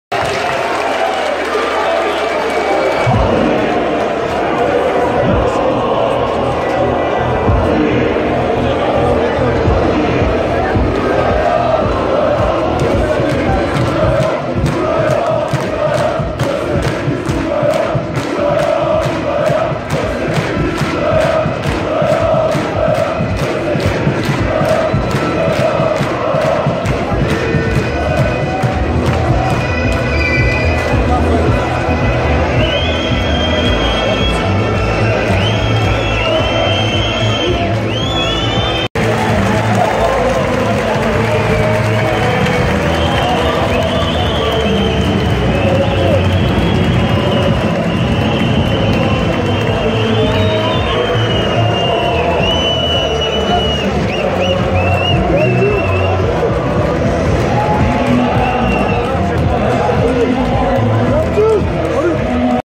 Osmanlıspor 0-2 Göztepe / mac sonu
Göztepemizin Osmanlıspor'u 2-0 yendiği maçın hemen sıcağı sıcağına sonu sizlerle. İyi seyirler